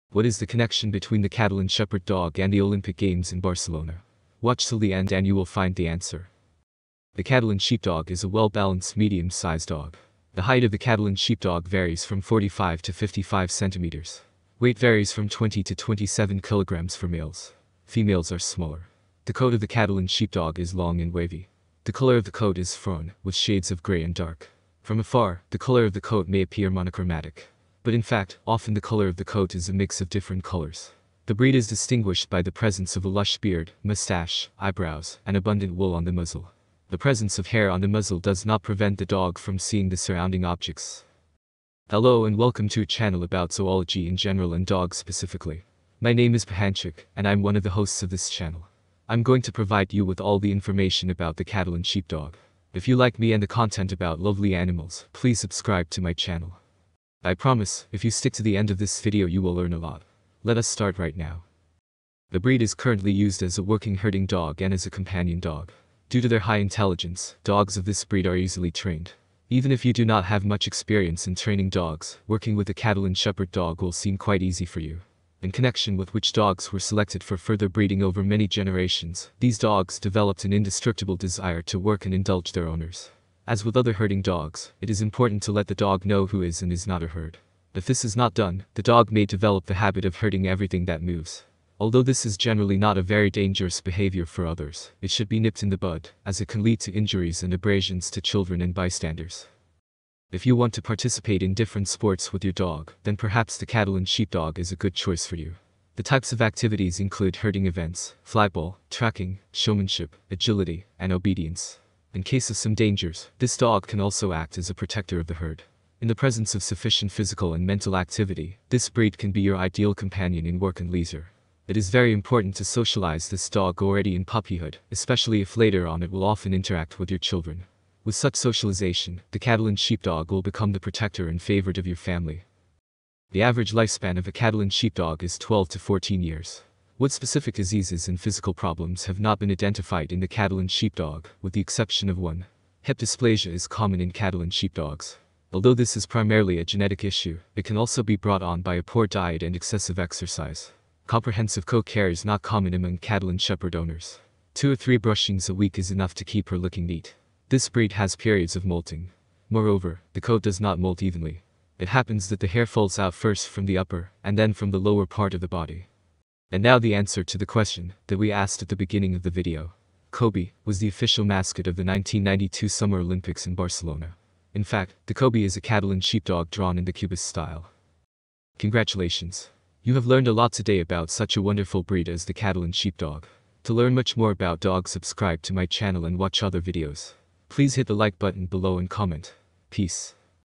Catalan Sheepdog 101. Basic facts about the breed.
Learn the basic and most essential facts about the Catalan Sheepdog. How famous Catalan Sheepdog is? Find out in this 2022 year video about the Catalan Sheepdog.

What is the Catalan Sheepdog's appearance? How can you describe the temperament of a Catalan Sheepdog? In general how healthy this dog breed is? Also, learn some fun facts about Catalan Sheepdogs.